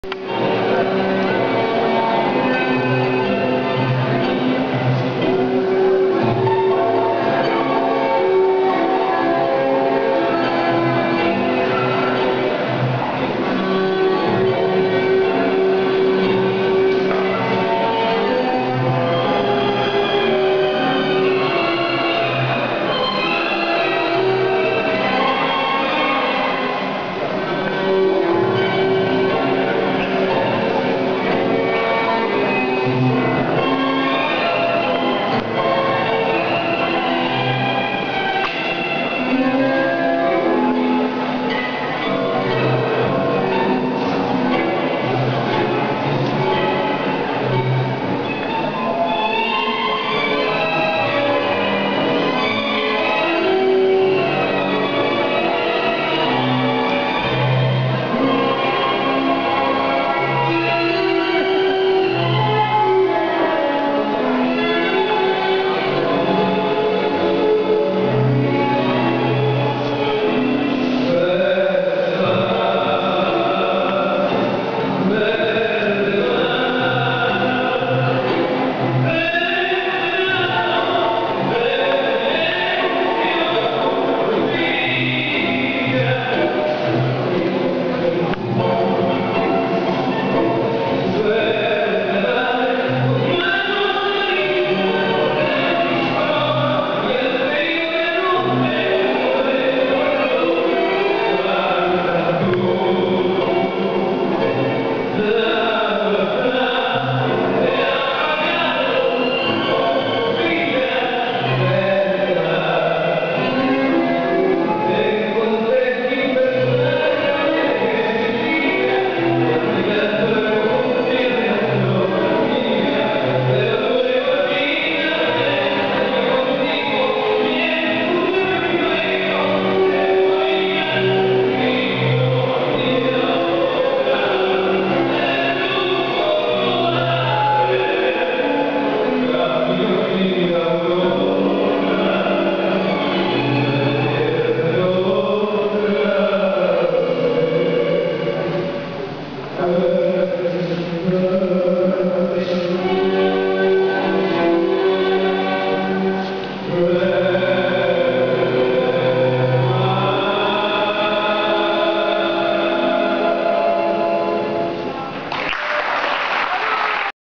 Didier et Anne-Marie au Festival de Tango Argentin de Château Thierry
Didier et Anne-Marie représentent Reims au festival de tango Argentin de Château Thierry